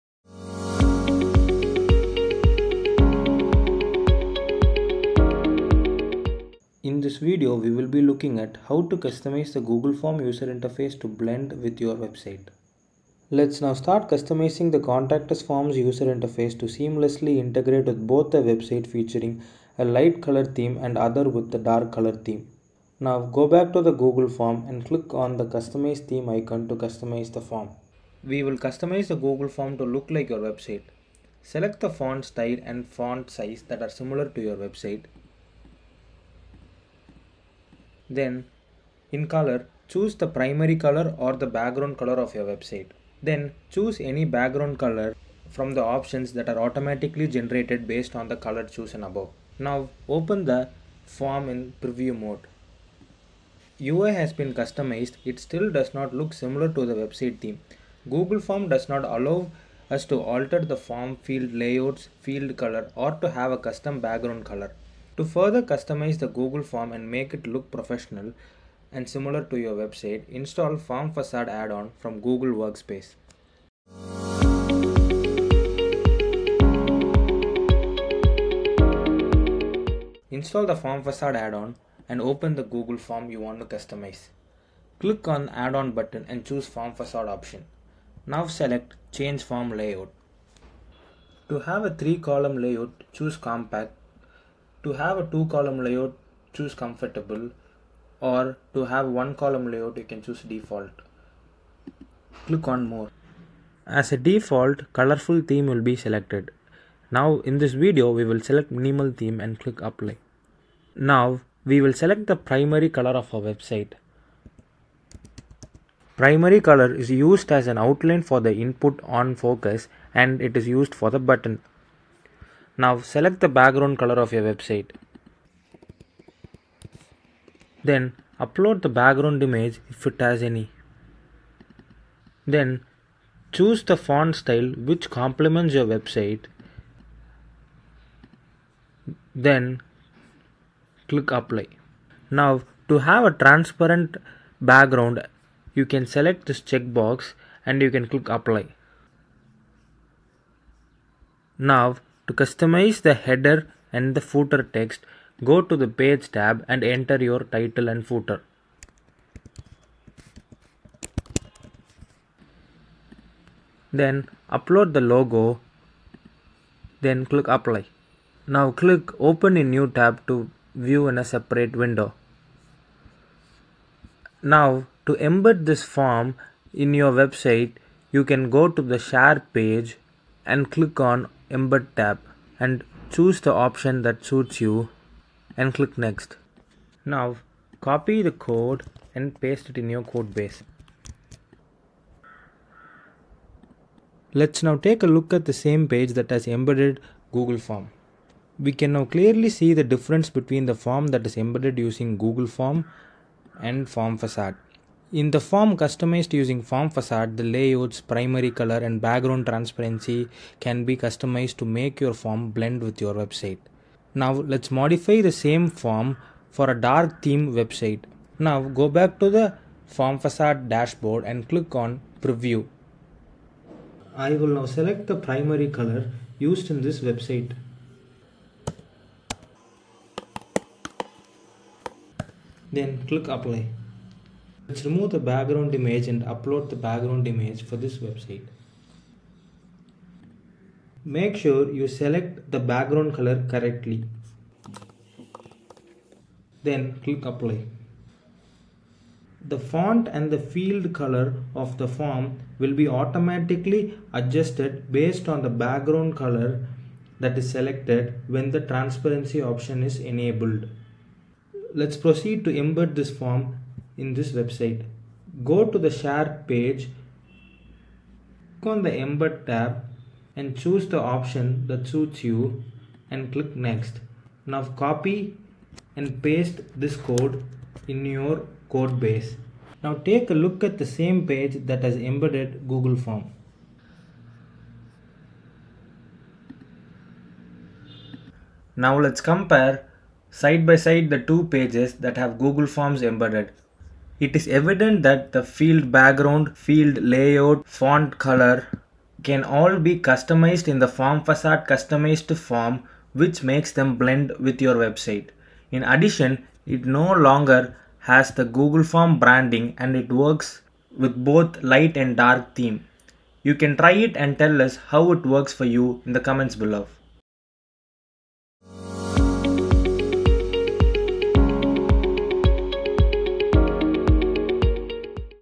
Customize Google Form and Embed with Your Website
In this comprehensive tutorial, discover how to elevate your Google Form's appearance to seamlessly align with your website's branding using Formfacade. Empower your forms with a professional touch!

🔍 Key Highlights:

Visual Consistency: Learn to match font styles, sizes, and colors to echo your website's design language.
Advanced Customization: Utilize Formfacade's features to tweak layouts and create a cohesive interface.
Transparent Integration: Seamlessly embed forms into your website without altering header and footer sections.
🖌️ Customization Process:
Explore step-by-step guidance on adapting your Google Forms using Formfacade. From color coordination to layout adjustments, ensure your forms resonate with your website's aesthetics flawlessly.

🛠️ Formfacade Features:
Unlock the potential of Formfacade for Google Forms. Tailor your forms to perfection, maintaining brand consistency and professional appeal effortlessly.

📐 Seamless Integration:
Discover the art of embedding customized forms into your website hassle-free. Implement Formfacade's features while keeping your website's header and footer intact.

🔗 Practical Implementation:
Get actionable insights to bridge the gap between your website's look and your Google Forms. Elevate your forms' visual appeal and usability for enhanced user engagement.

🌟 Elevate Your Forms Today! 🌟
Access this tutorial to transform your Google Forms into sleek, website-matching interfaces. Enhance user experience and reinforce your brand identity seamlessly.

🔔 Don't miss out! Let's refine your forms for maximum impact! 🚀